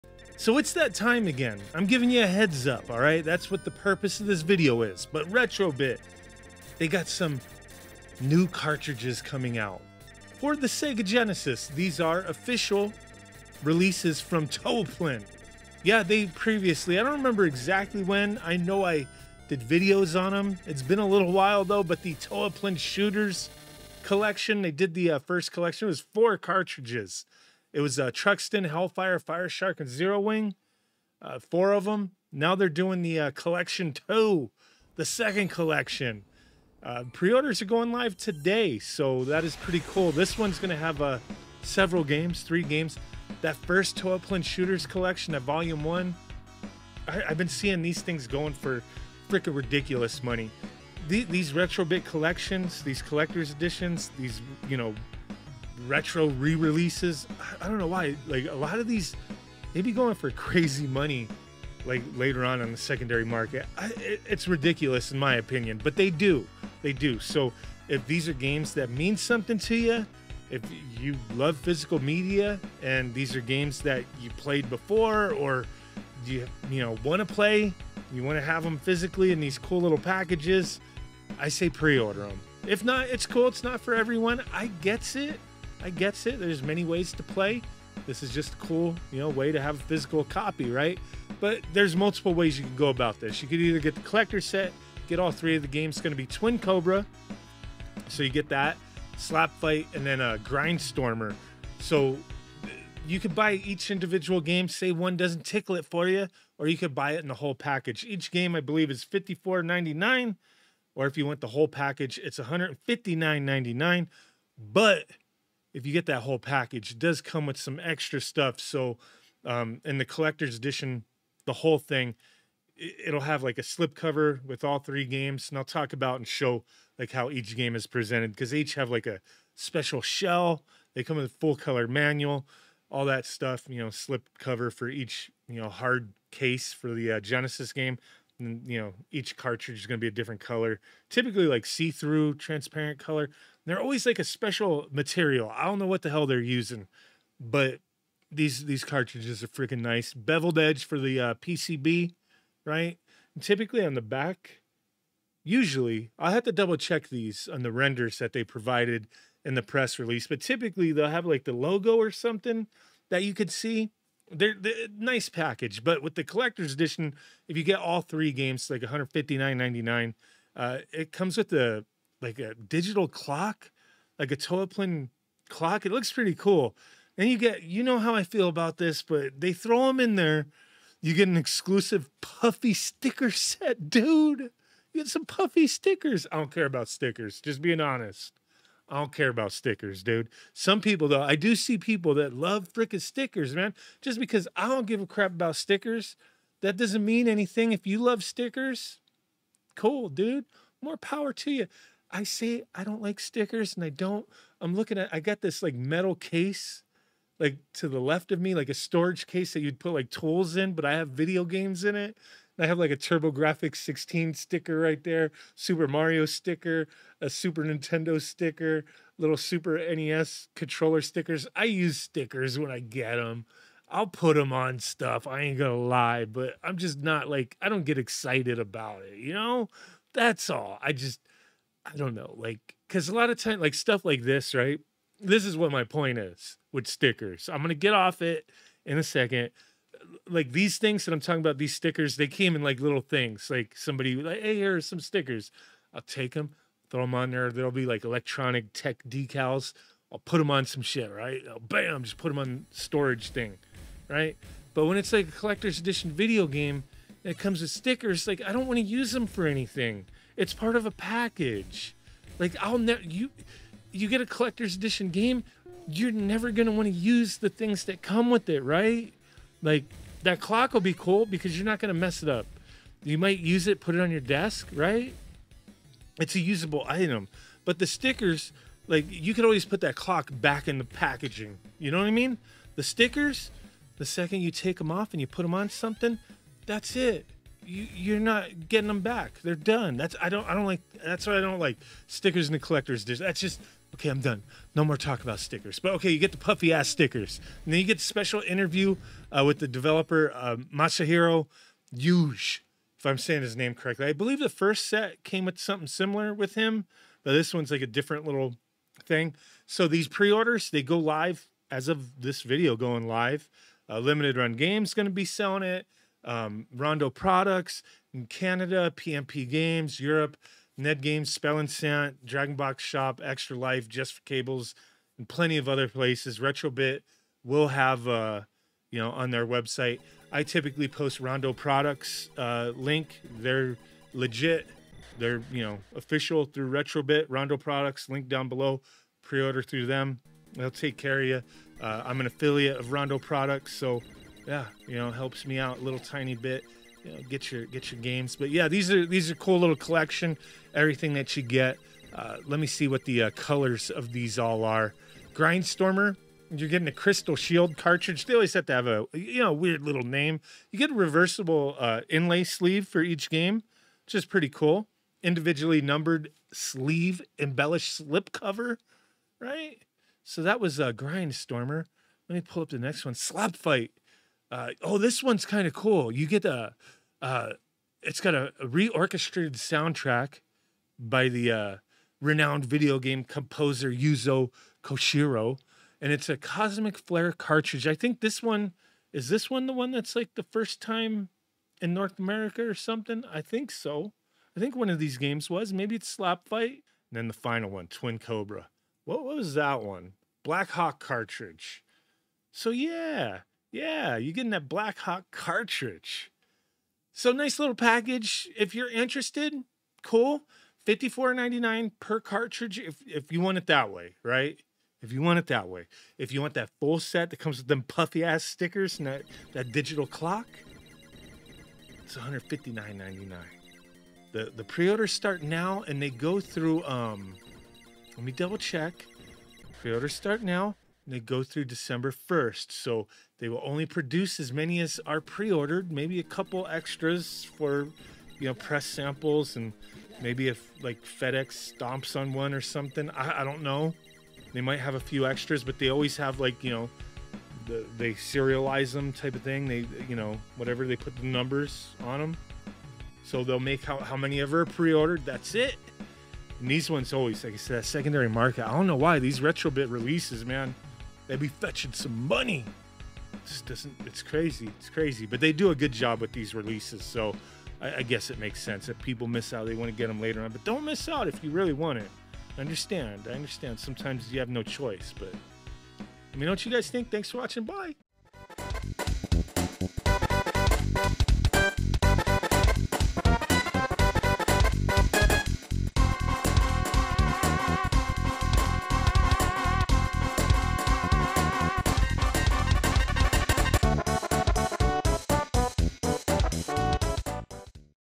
This is AWESOME! NEW SEGA Genesis Cartridges! More Toaplan Retro Re-Releases!
Toaplan Shooters collection 2 is coming from Retro-bit!